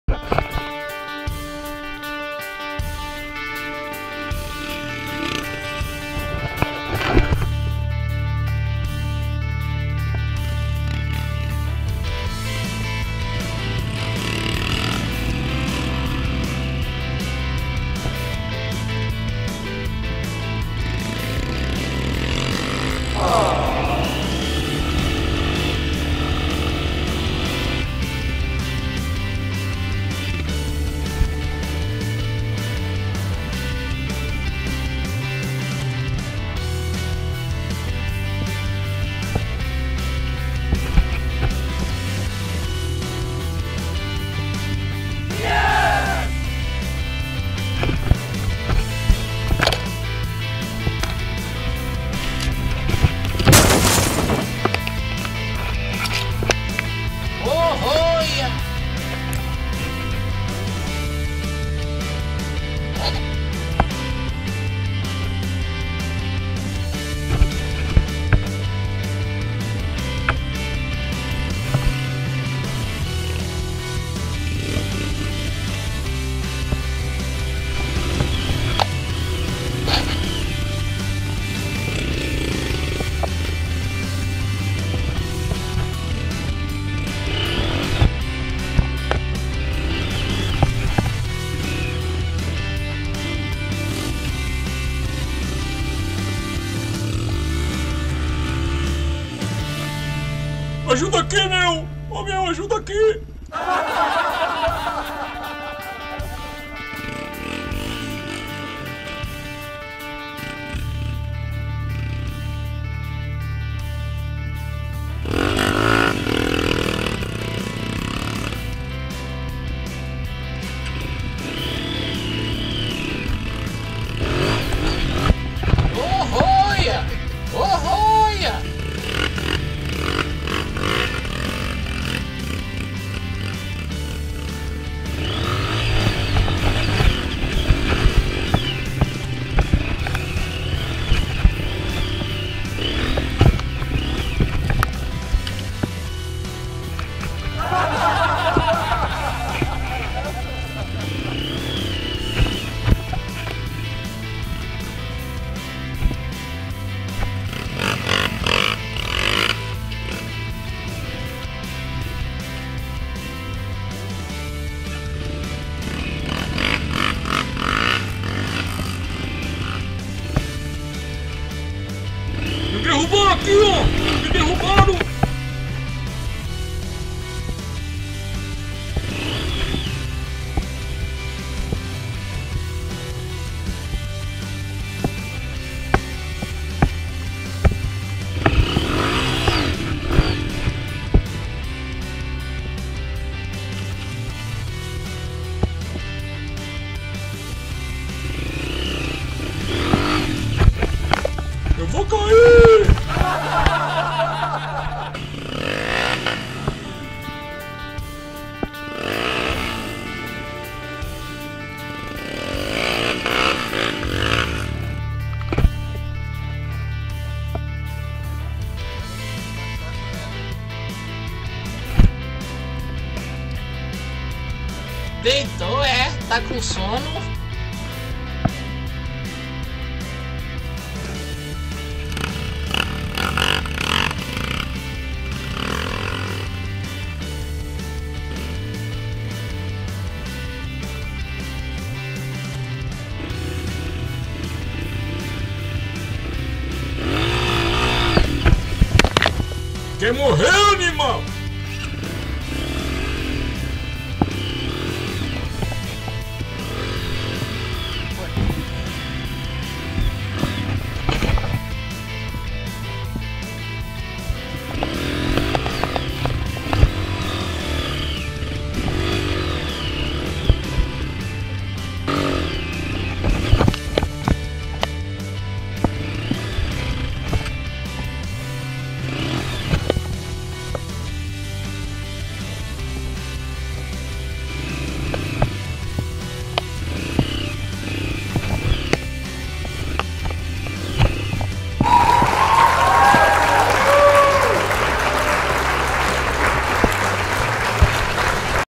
Trilha de iniciantes no Riachinho ao contrario é certeza de tombos
Trilha feita lá por 2018 com o Ricardinho e o Jesus
Quer ver como é uma trilha de motos de iniciantes ? Assista o vídeo e veja nossas motos para trilha, pegue algumas dicas de iniciantes e aprecie muitos tombos.